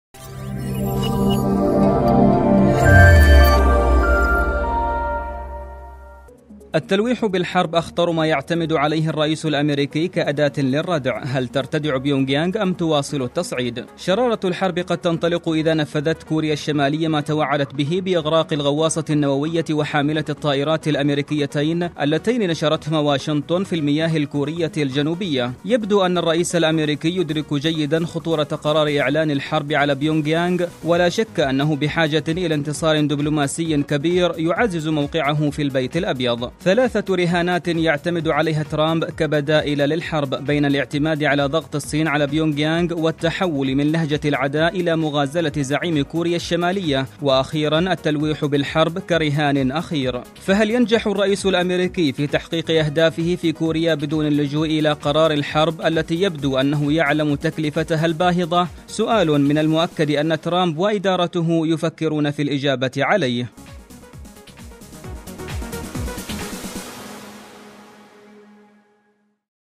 علوم الشبيبة - ماهي رهانات ترامب لتجنب الحرب في كوريا ؟
ماهي رهانات ترامب لتجنب الحرب في كوريا ؟